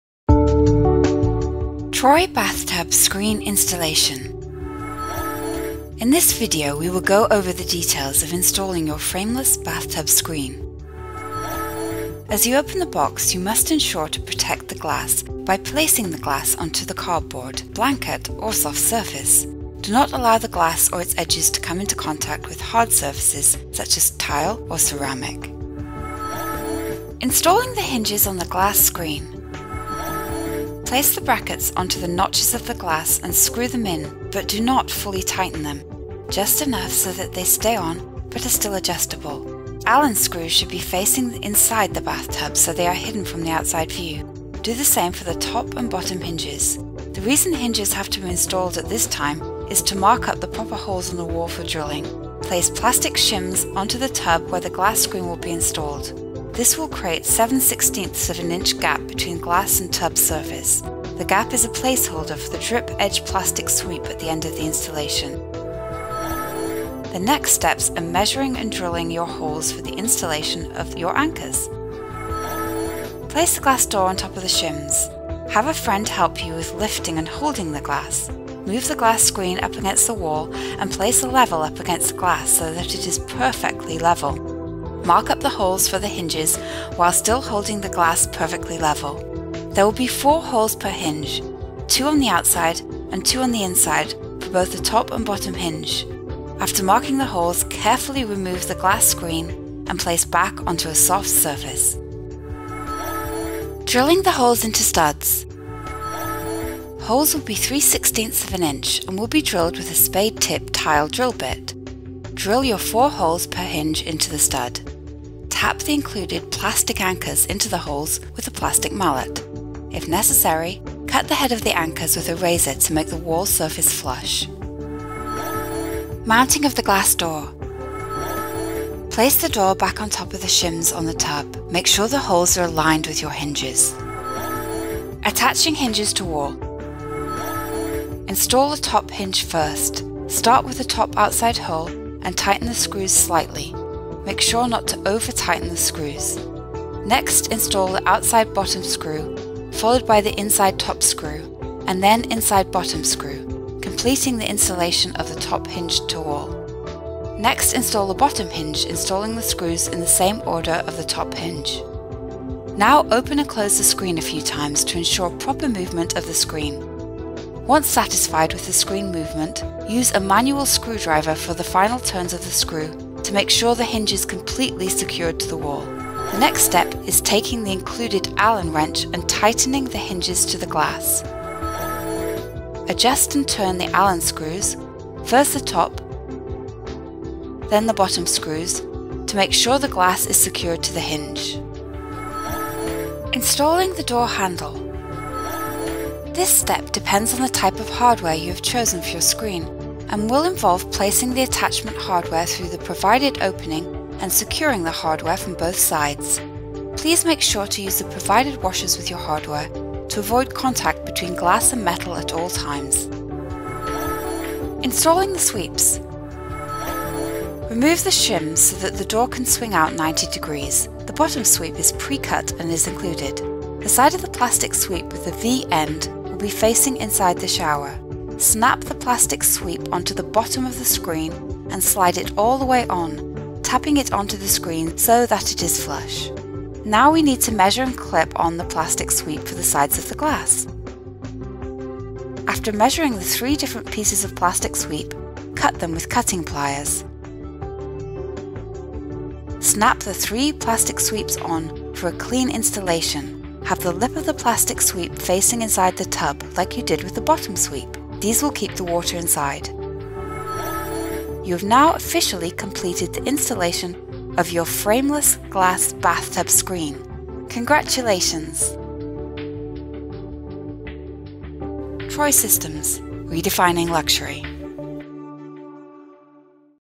How To Install a Bathtub Screen | DIY Installation Instructions TroySystems
Troy Systems Bathtub Screen Installation | Print Instructions

As you open the box you must ensure to protect the glass by placing the glass onto the cardboard, blanket or soft surface. Do not allow the glass or its edges to come in contact with hard surfaces such as tile or ceramic.

Installing the Hinges on the Glass Screen:

Place the brackets onto the notches of the glass, and screw them in but do not fully tighten them -- just enough so that they stay on but are still adjustable. Allen screws should be facing inside the bathtub, so they are hidden from the outside view. Do the same for the top and bottom hinges.
The reason hinges have to be installed at this time is to mark up the proper holes on the wall for drilling.

Place plastic shims on to the tub where the glass screen will be installed. This will create a 7/16ths of an inch gap between glass and tub surface. This gap is a place holder for the drip edge plastic sweep at the end of the installation.

The next steps are Measuring and Drilling Your Holes for the installation of your anchors.
Place the glass door on top of the shims.
Have a friend help you with lifting and holding the glass.
Move the glass screen up against the wall and Place a level up against the glass so that it is perfectly level.
Mark up the holes for the hinges while still holding the glass perfectly level.
There will be 4 holes per hinge - 2 on the outside and 2 on the inside for both the top and bottom hinge.
After marking the holes, carefully remove the glass screen, and place back onto a soft surface.

Drilling the Holes into Studs:

Holes will be 3/16th of an inch and will be drilled with a spade tip tile drill bit.
Drill your 4 holes per hinge into the stud.
Tap the included plastic anchors into the holes with a plastic mallet.
If necessary, cut the head of the anchors with a razor to make the wall surface flush.

Mounting of the Glass Door:

Place the door back on top of the shims on the tub.
Make sure that the holes are aligned with your hinges.

Attaching Hinges to Wall:

Install the top hinge first. Start with the top outside hole and tighten the screw slightly, Make sure not to over tighten the screws.

Next install the outside bottom screw followed by inside top screw and then inside bottom screw -- completing the installation of the top hinge to the wall.

Next install the bottom hinge installing the screws in the same order of the top hinge.
Now open and close the screen a few times to ensure proper movement of the screen.
Once satisfied with the screen movement, use a manual screw driver for the final turns of the screw to make the hinges completely secured to the wall.

The next step is taking the included Allen wrench and Tightening the Hinges to the Glass.
Adjust and turn the Allen screws (first the top, then the bottom screws), to make sure the glass is secured to the hinge.

Installing the Door handle:

This step depends on the type of hardware you have chosen for your screen and will involve placing the attachment hardware through the provided opening and securing the hardware from both sides. Please make sure to use the provided washers with your hardware to avoid contact between glass and metal at all times.
Installing the Sweeps:
Remove the shims so the door can swing out 90 degrees.
The bottom sweep is pre-cut and is included.
The side of the plastic sweep with the V end will be facing inside the shower.
Snap the plastic sweep onto the bottom of the screen and slide it all the way on, tapping it onto the screen so that it is flush.
Now we need to measure and clip on the plastic sweep for the sides of the glass.
After measuring the 3 different pieces of plastic sweep, cut them with cutting pliers.
Snap the three plastic sweeps on, for a clean installation. Have the lip of the plastic sweep facing inside the tub, like you did with the bottom sweep. These will keep the water inside.

You have now officially completed the installation of your Troy Systems Frameless Glass Bathtub Screen.
Congratulations!